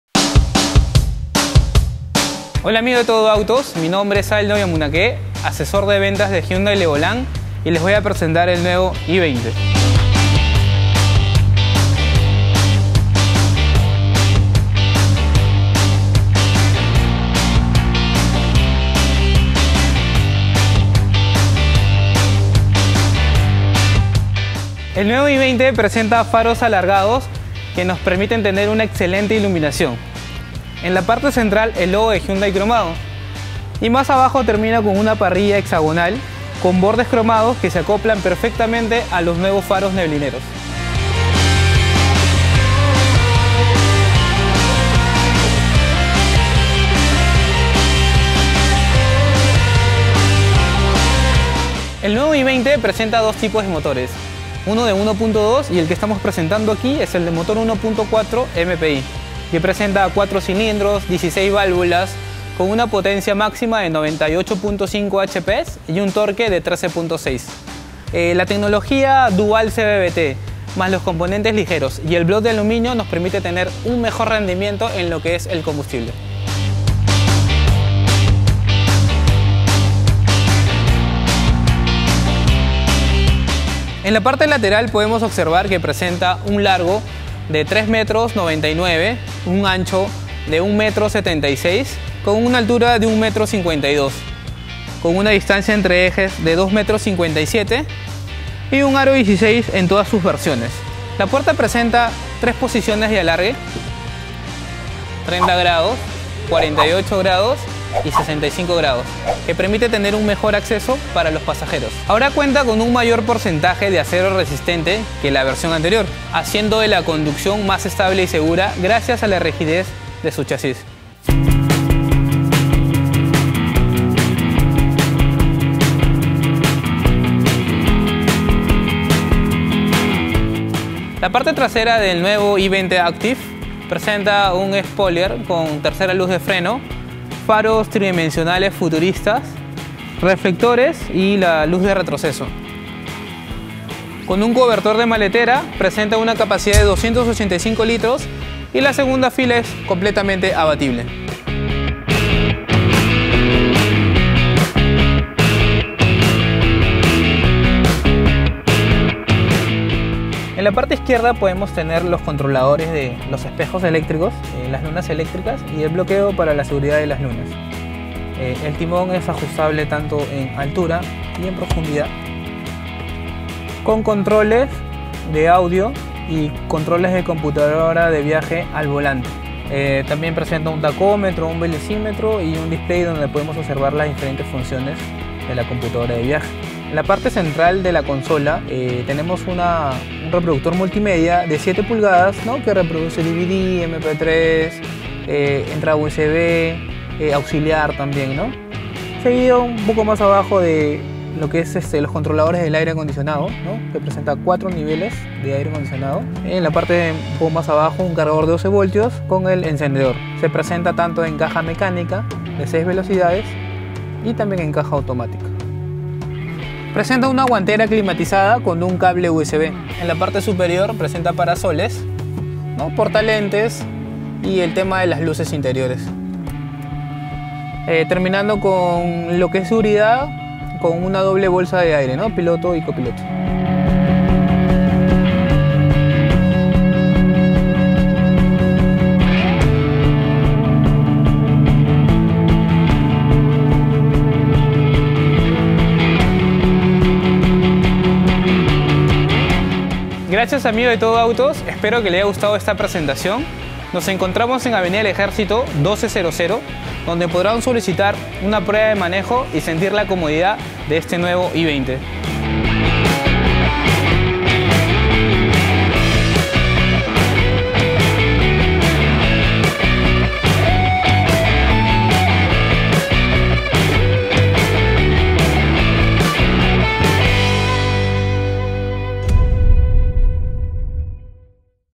Nuevo Hyundai i20 | TODOAutos.pe
Hyundai Le Volant presenta el revonado i20, cuenta con la parrilla hexagonal ya habitual de Hyundai, amplio capó, faros alargados, alerón posterior, diseño de cabina centrada en el conductor y el confort de los ocupantes, entre otras características.